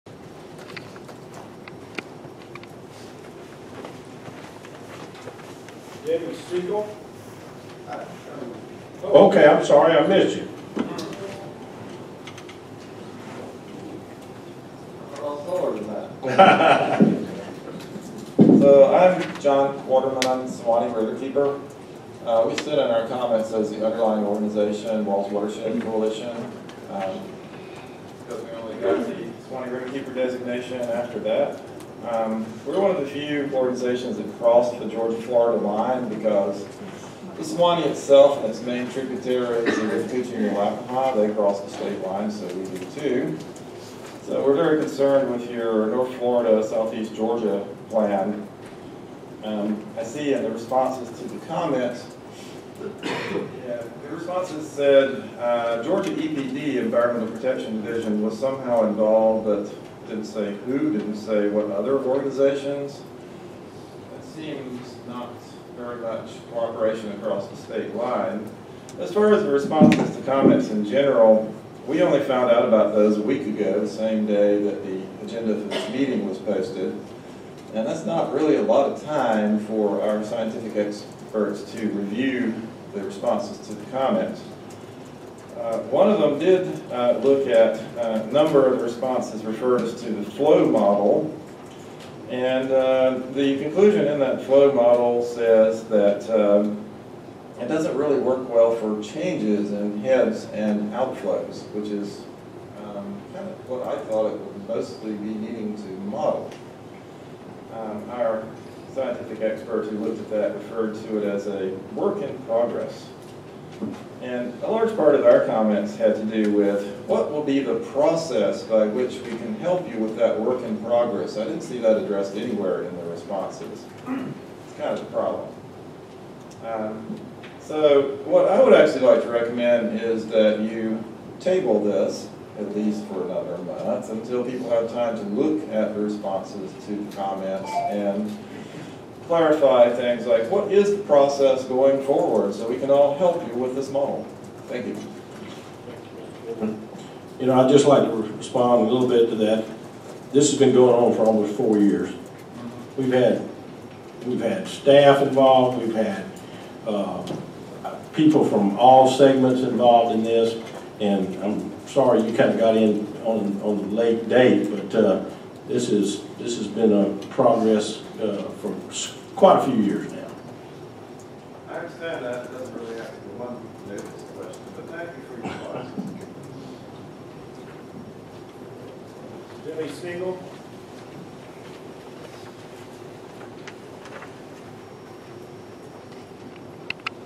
Model inadequate, notice, process, table --John S. Quarterman, Suwannee Riverkeeper @ 2017-01-17
NFRWSP Plan passed at joint SRWMD-SJRWMD Board Meeting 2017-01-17